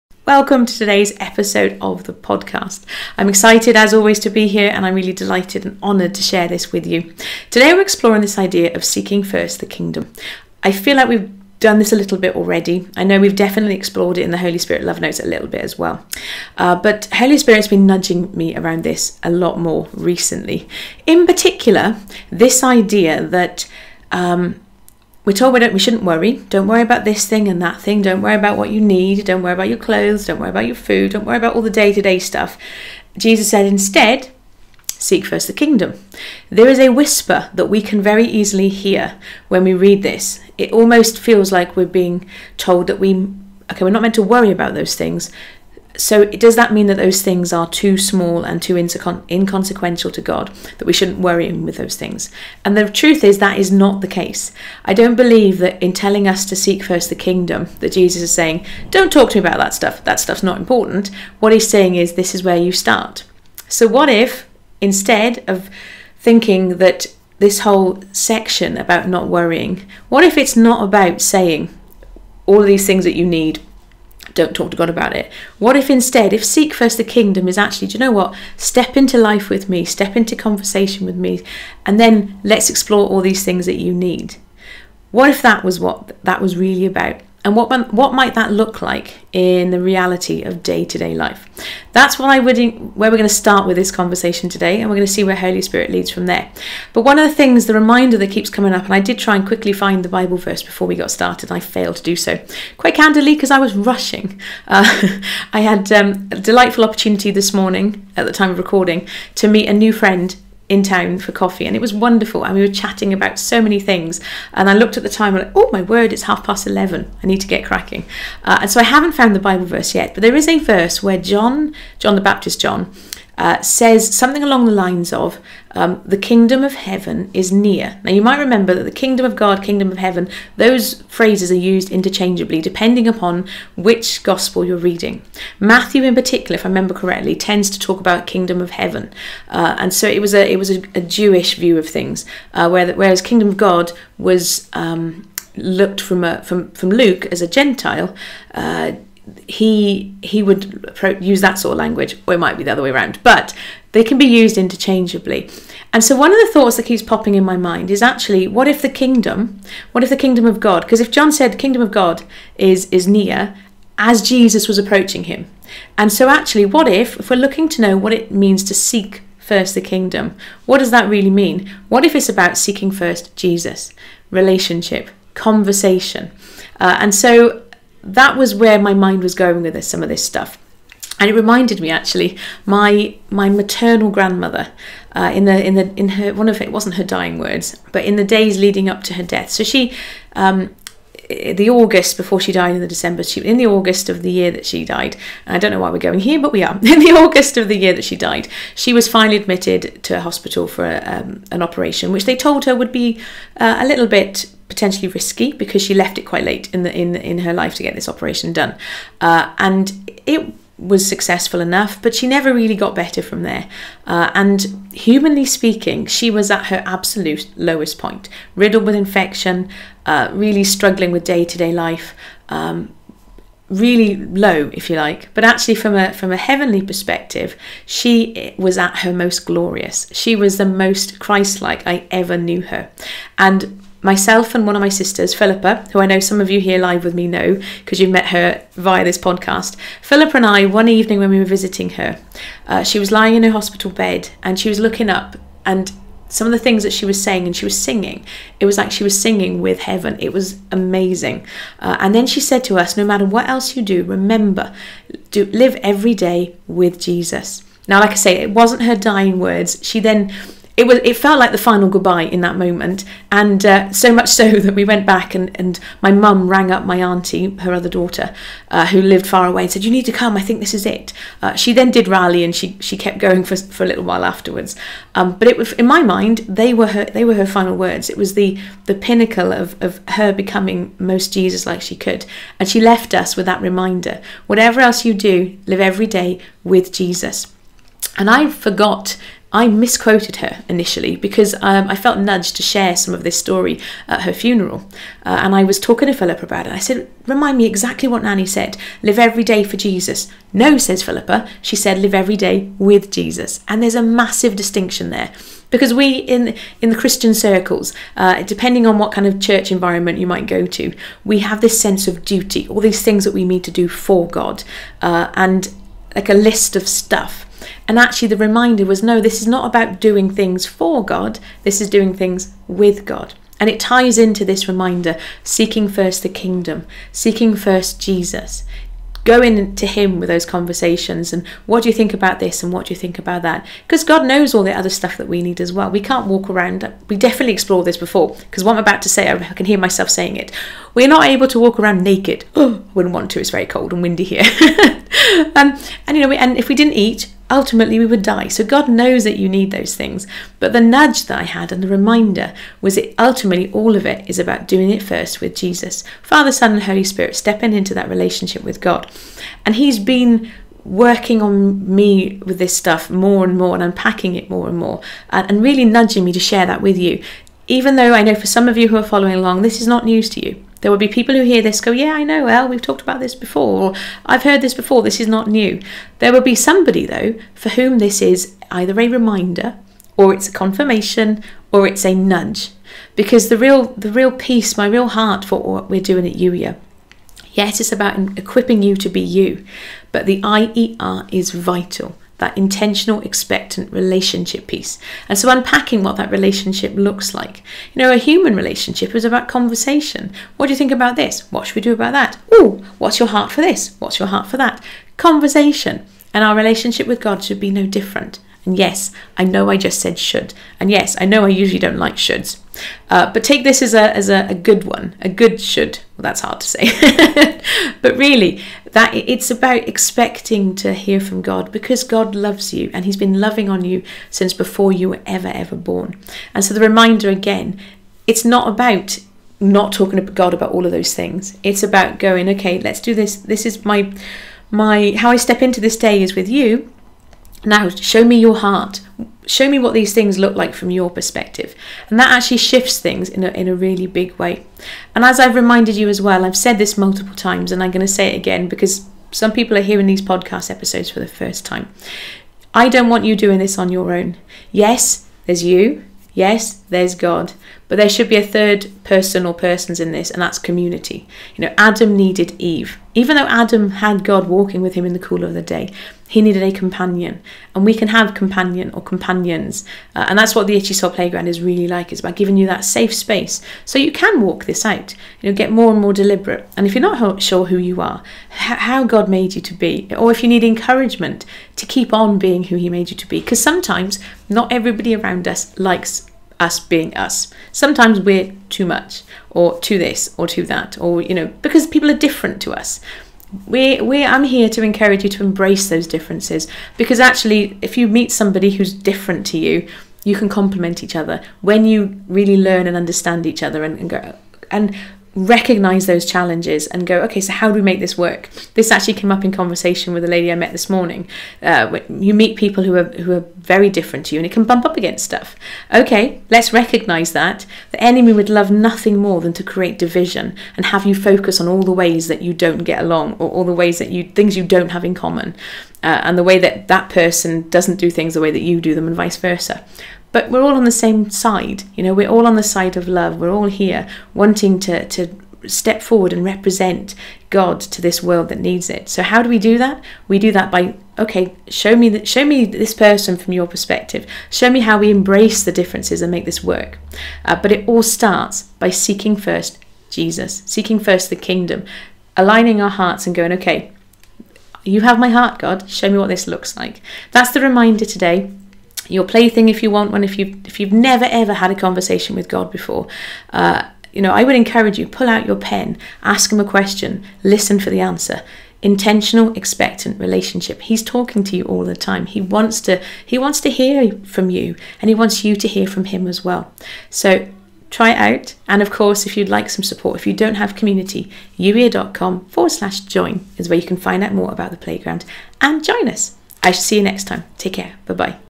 'Seek first the Kingdom' was always about My heart
Welcome to a brand new episode of the podcast! Today we're exploring one of the most well-known sound bites Jesus ever delivered ...

"But seek first his kingdom and his righteousness, and all these things will be given to you as well. " Matthew 6:33

Is this a call to not think about anything other than God? Sounds kind of impossible to me. There's a lot of "real life" stuff going on around us! What if, instead, Jesus was exploring how we move through life - while we do the "real life" stuff?

That's what we're considering today. And it comes with the reminder that this exploration is a lot more fun when you do it as God intended - in community. If you don't already have a safe space to explore being Youier (You + Intentional, Expectant, Relationship) come and find out more about the Itchy Soul Playground and join us